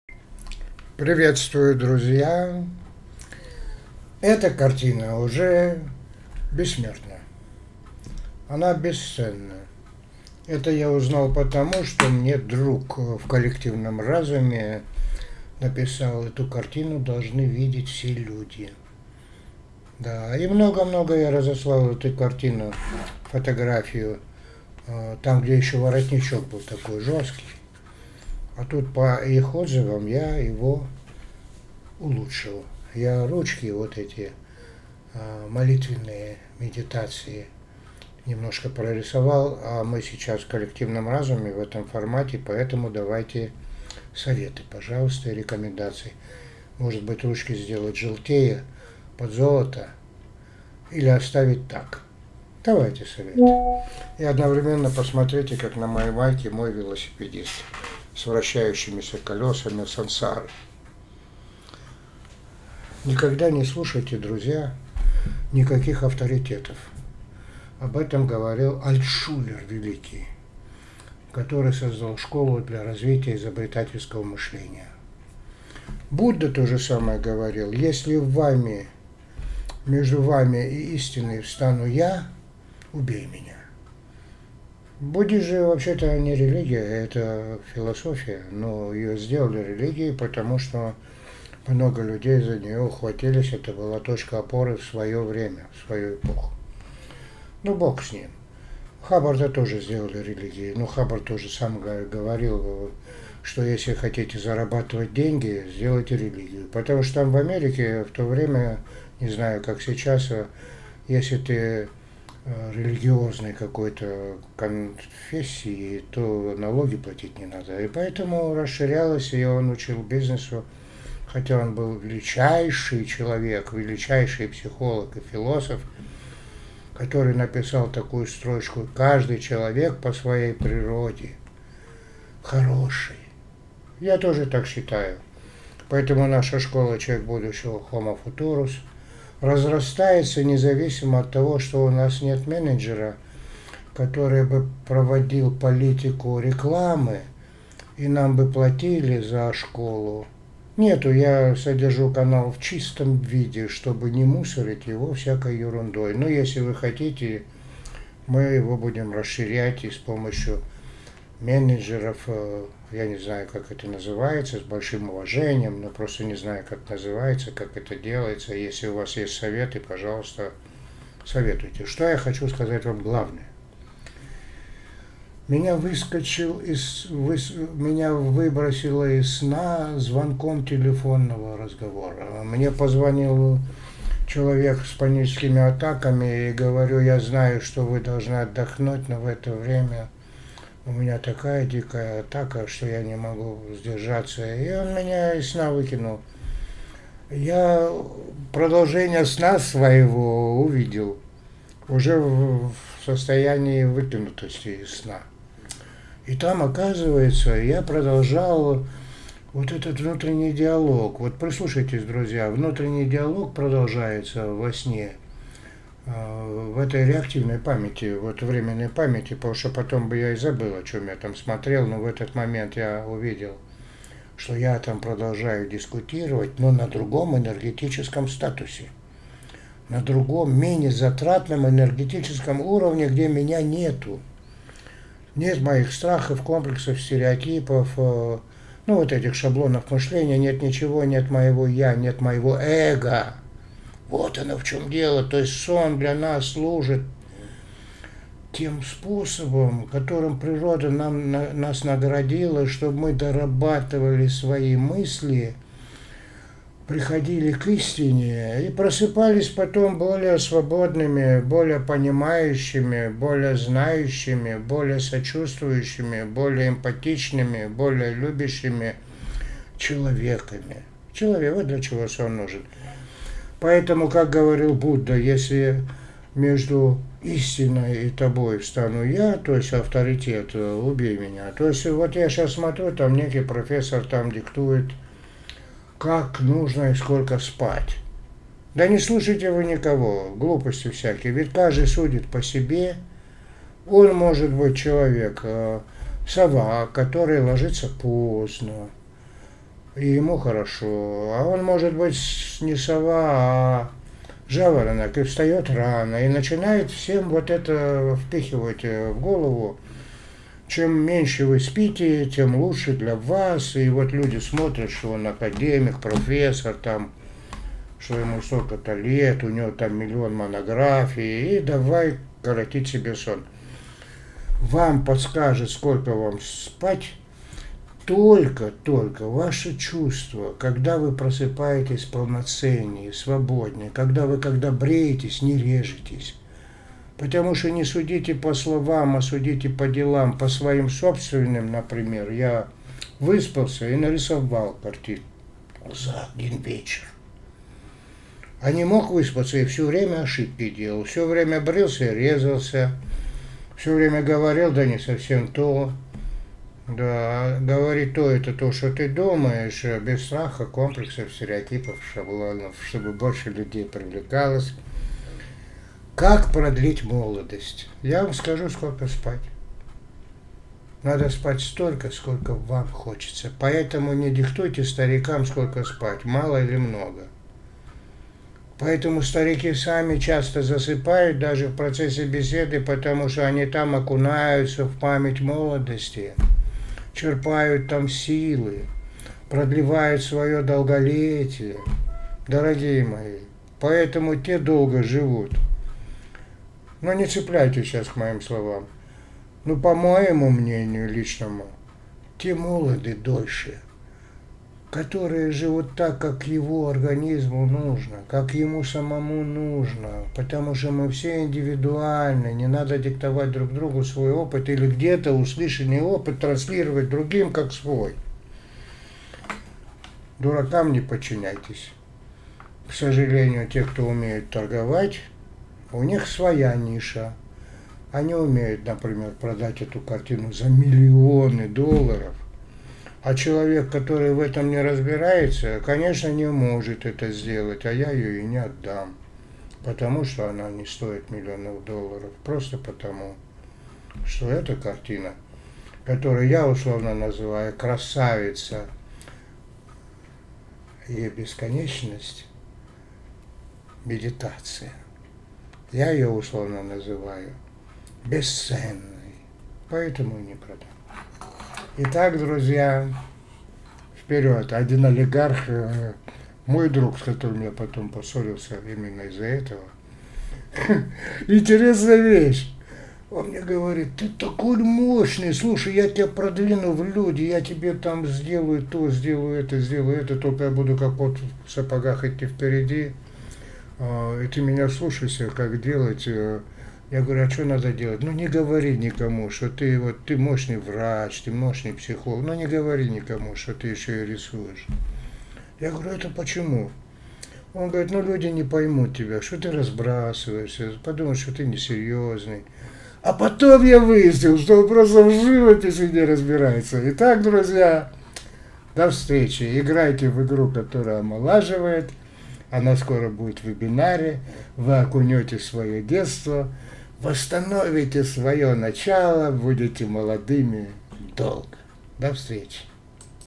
Сколько спать, как включить силу и красоту молодости, жить долго
Доктор Хасай Алиев рассказывает как быть вечно молодым, крсивым и созидающим, дает примеры и критерии для практической проверки и использования